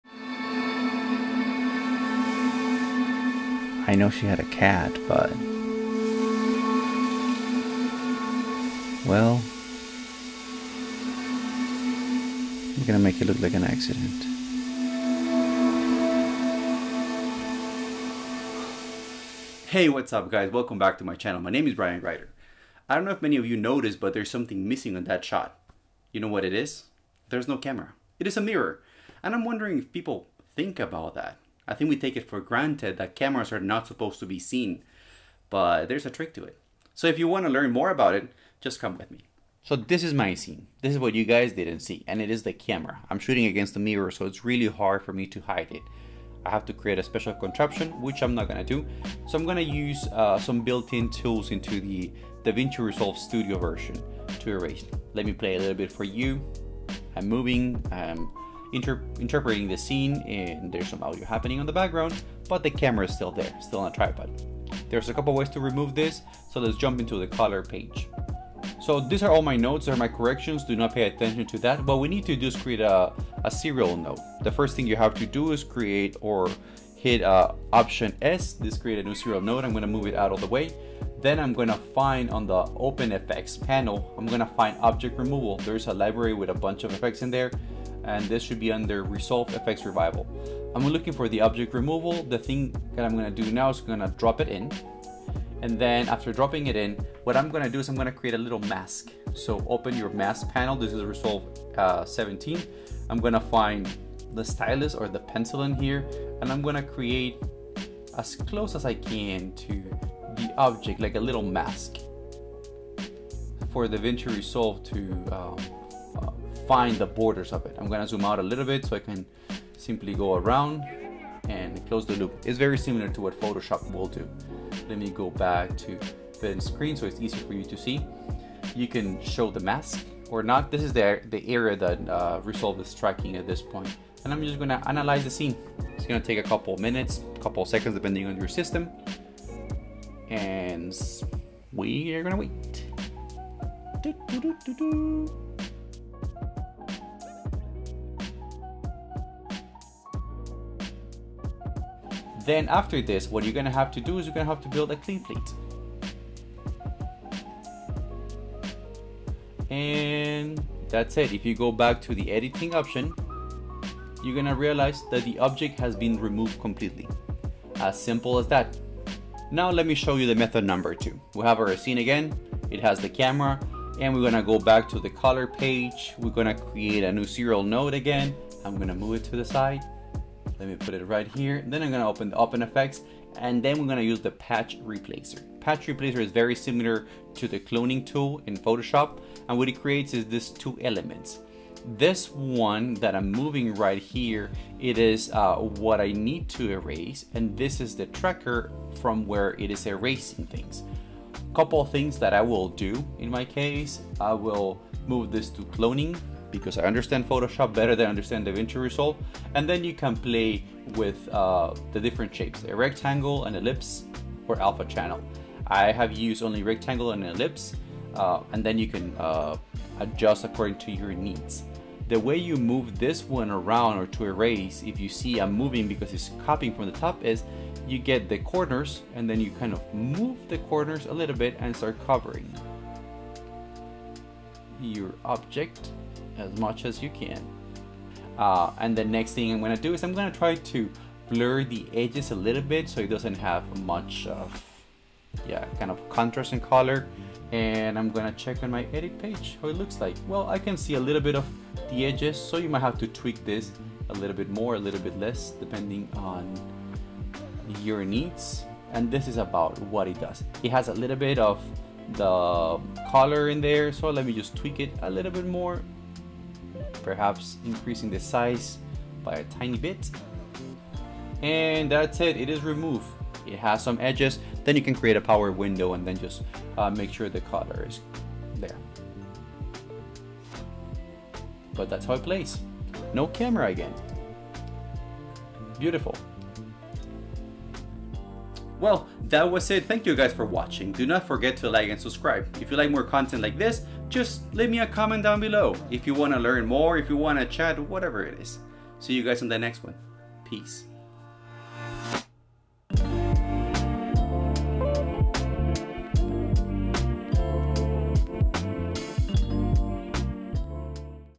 The "Mirror Shot." Removing unwanted objects in Davinci Resolve
Do you have a BMPCC, got the Davinci Resolve Studio license, shot something cool, and now need to get rid of n wanted object o the scene? Or are you shooting in front of a mirror, and your camera is visible? Let me show you two ways to get rid of unwanted objects in your videos!